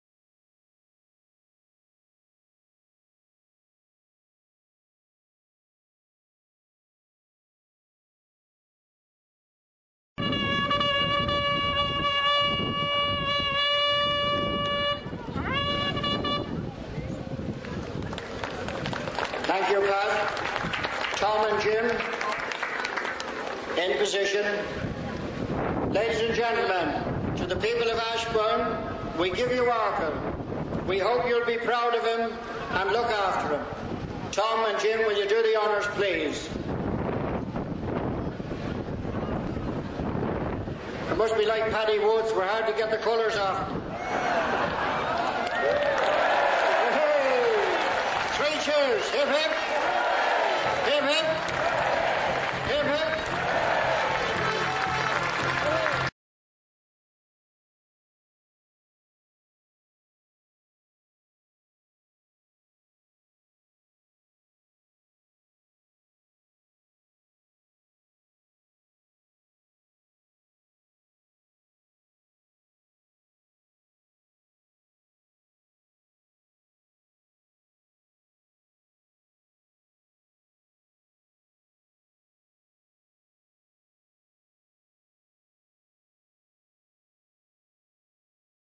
Arkle's Coming Home
The legendary racehorse, Arkle was commemorated with a permanent monument in Ashbourne,  Co Meath Ireland when a bronze sculpture by  Emma McDermott of the champion steeplechaser and his regular jockey, Pat Taaffe was unveiled on Saturday.19/4/2014 to a huge crowd in Ashbourne.
Short  clip of the  moment the statue was unveiled and photos of this beautiful sculpture